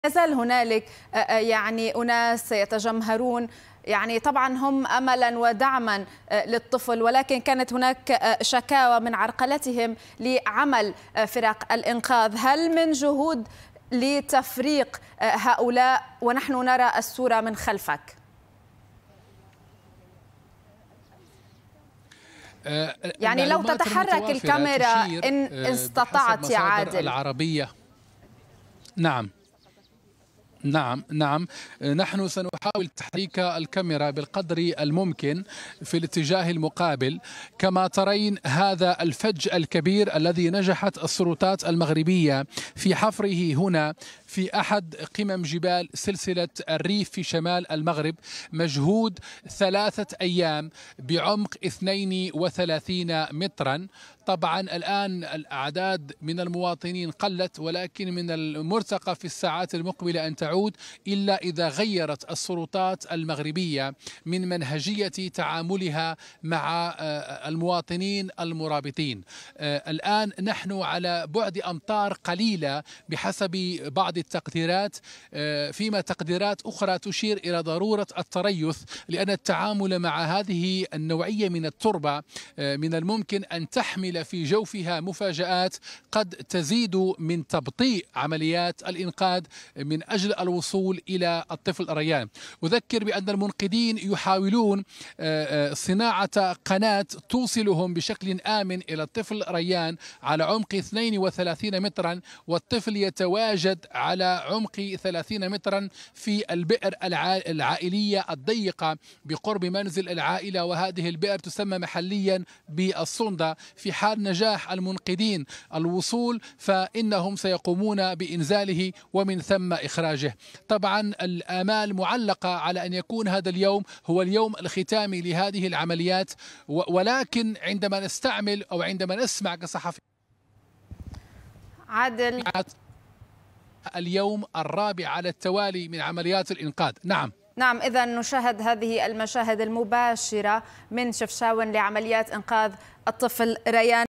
مشاهد مباشرة للعربية من شفشاون لعمليات إنقاذ الطفل ريان
مشاهد مباشرة لـ العربية من شفشاون لعمليات إنقاذ الطفل_ريان